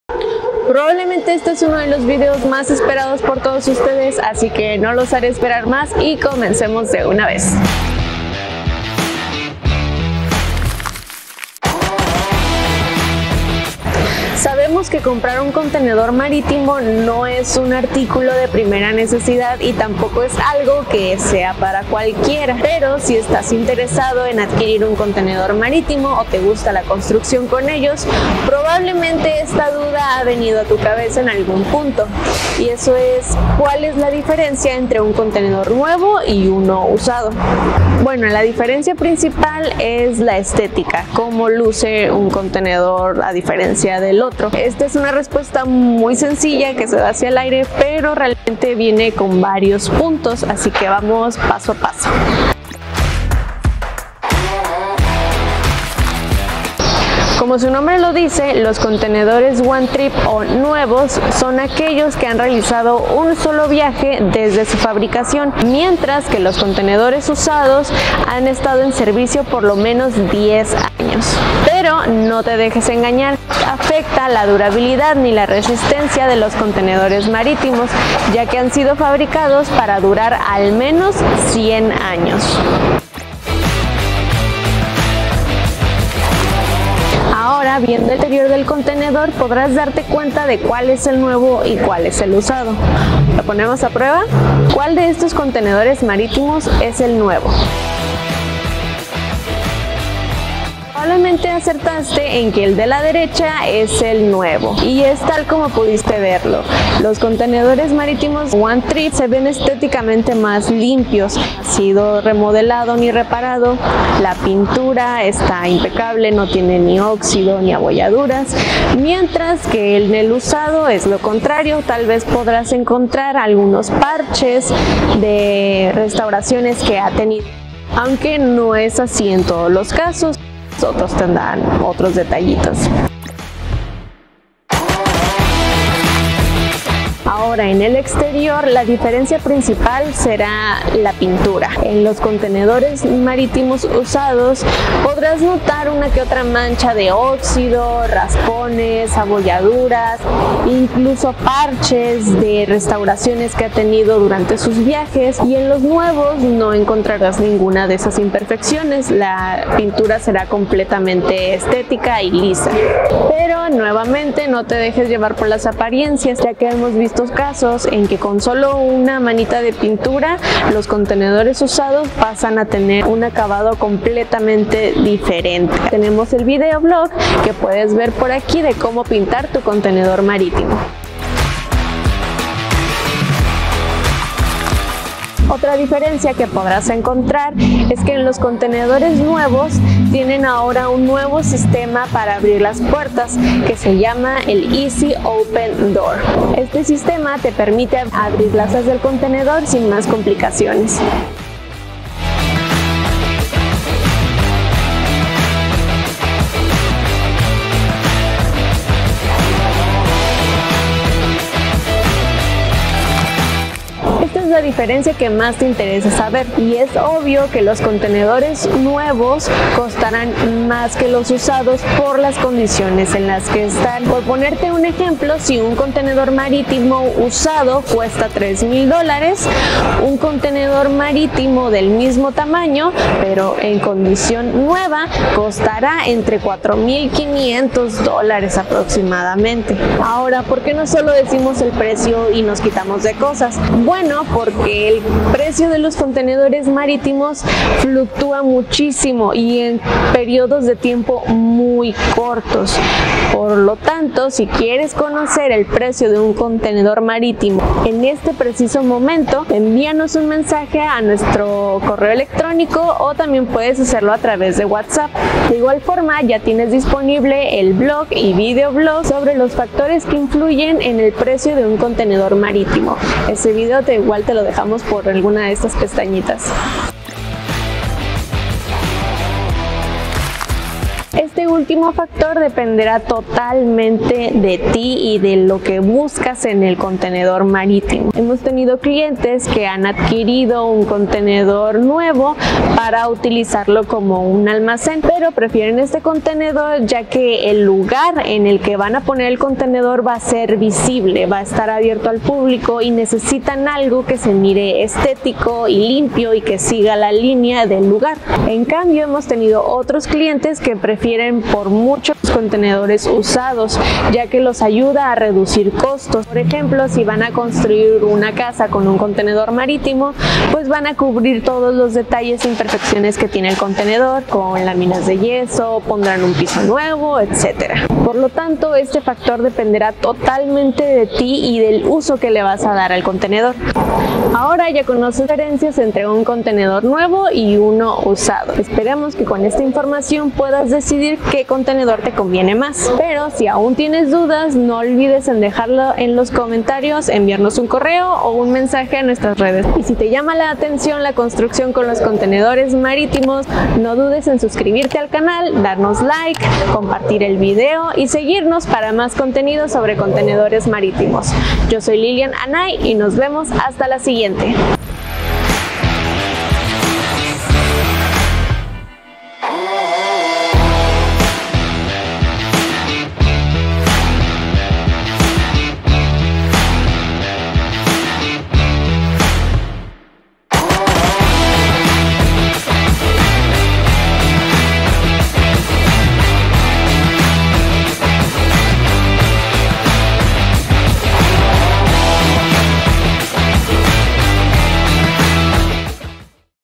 CONTENEDORES NUEVOS vs USADOS | CONTENEDORES MAS+
¿Qué tipo de contenedor marítimo te conviene comprar? Esta es una duda recurrente en temas de construcción con contenedores marítimos, esperamos que con esta guía puedas tomar la mejor decisión para tu proyecto.

¡Te dejamos el blog por acá!

🎵 Música por